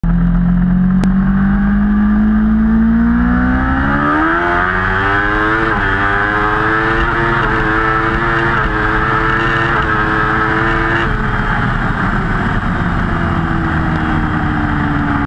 Kawasaki ZX10R Shift Light Testing
Shift light is a Boost By Smith shift light setup with Autoshift. The light that came with wasn't the most aesthetically pleasing so swapped it out for a white dual LED strip under the tach. As you can see it is very bright at night and still visible during the day.